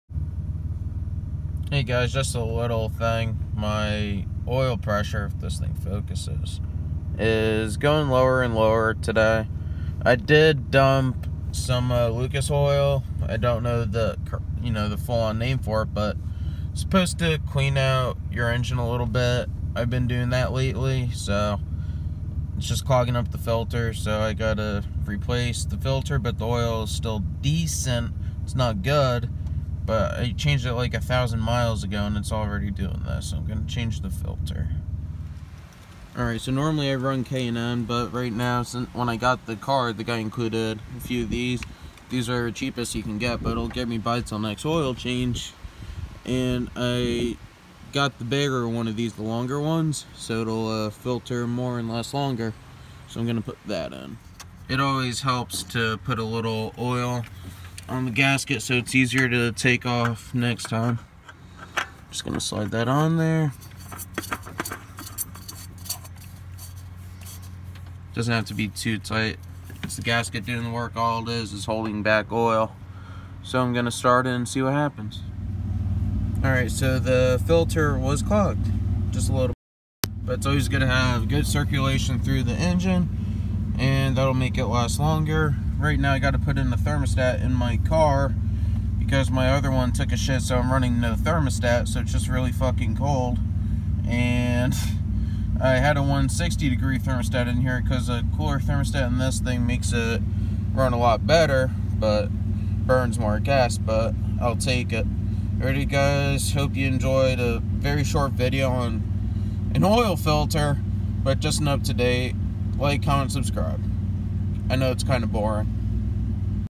Clogged Oil Filter
A clogged oil filter isn’t good for the engine...the bypass opens in the filter so it gets some decent flow but the filtering part is no longer filtering so it’s just running debris  through the engine, until I replaced the filter. Not the best filter but will get the job done until next oil change👌🏻